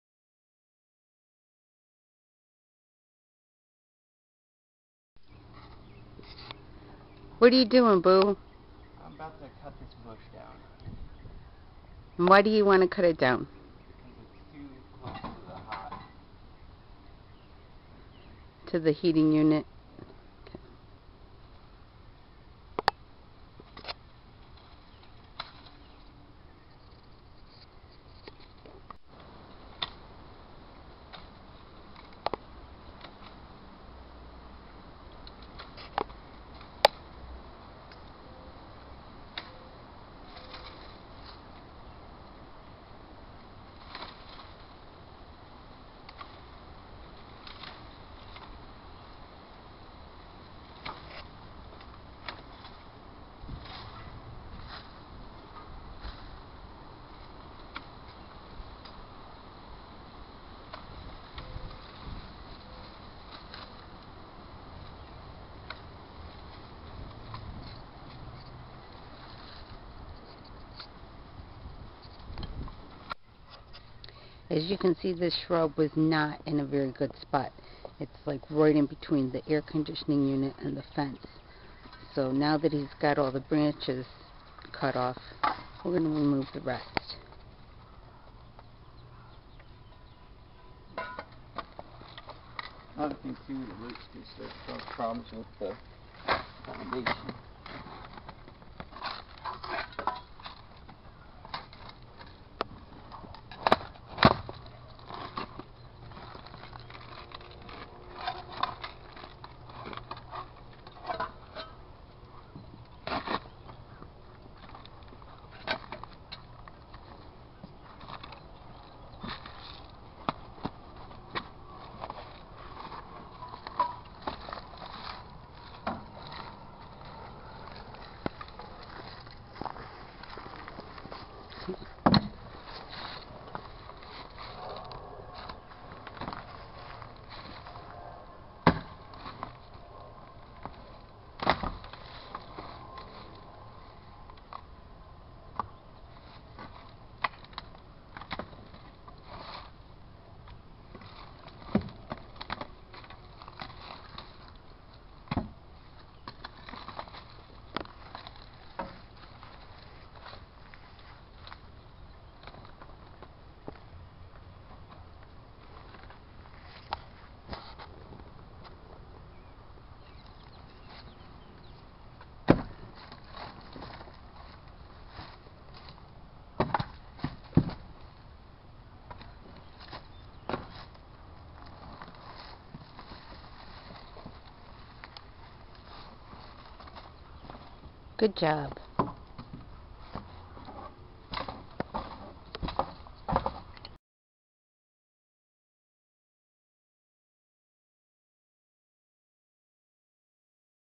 How to Remove a Shrub from Our Home Tips
It shows you how to remove a shrub or bush. Please like or share Thank you. remove shrub take out shrub bush # removal